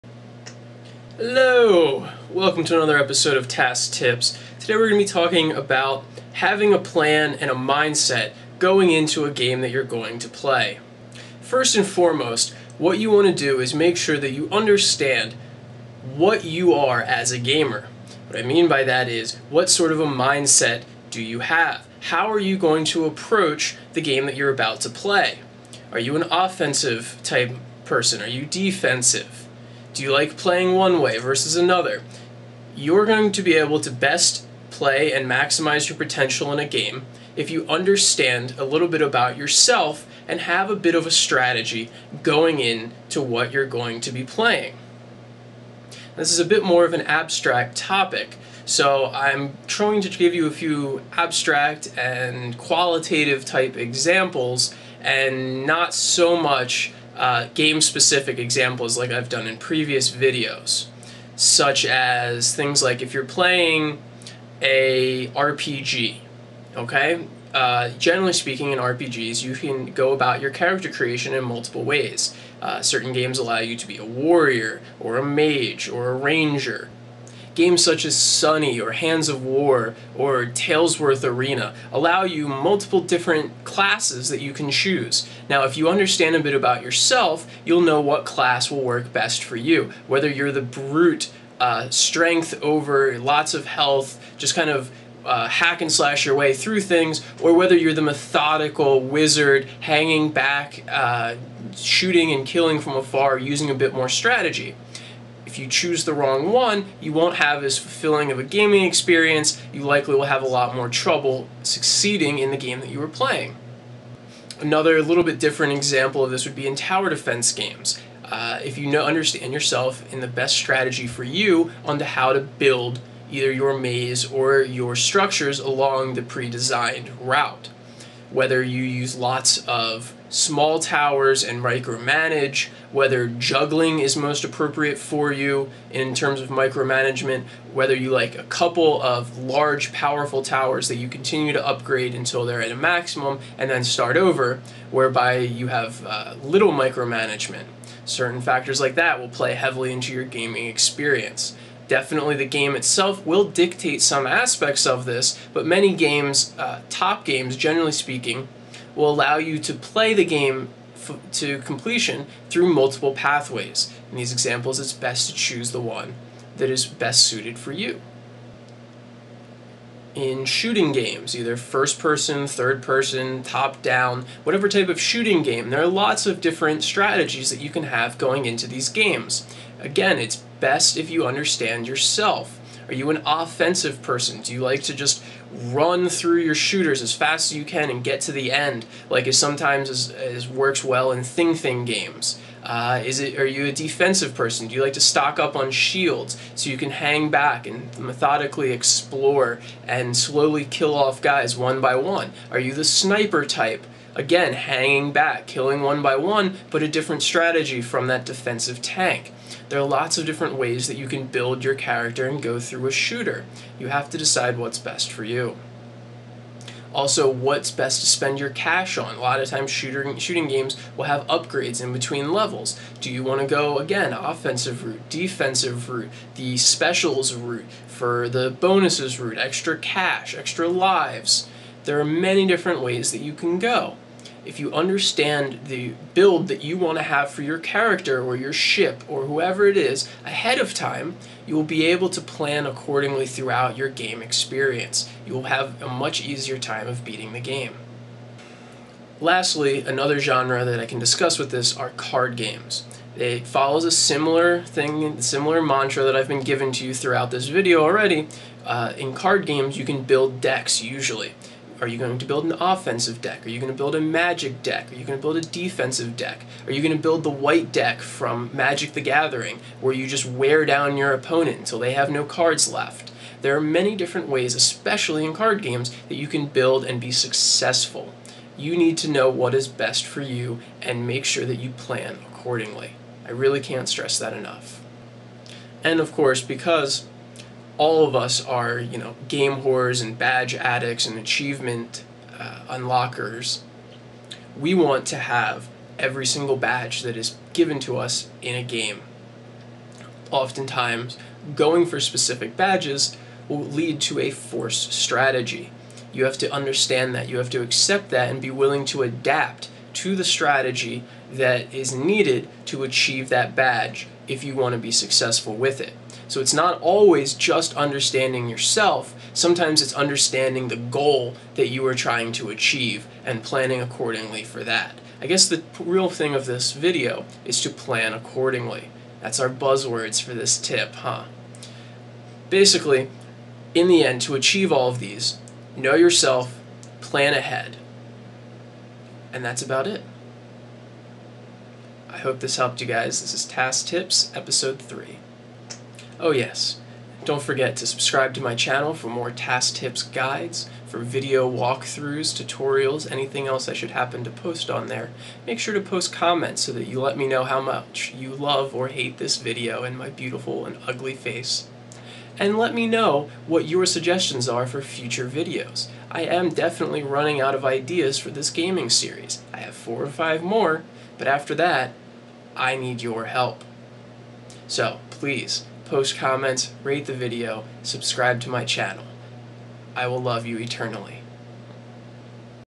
Tass Tips Episode 3 - Know Yourself & Plan Accordingly
Howdy.  I'm Tass.  This is the 3rd episode in my general gaming tips series.  Today's topic is about knowing yourself, understanding yourself as a gamer, and planning your strategy accordingly.  I hope it helps!

If you have any ideas for future episodes, please post them as a comment.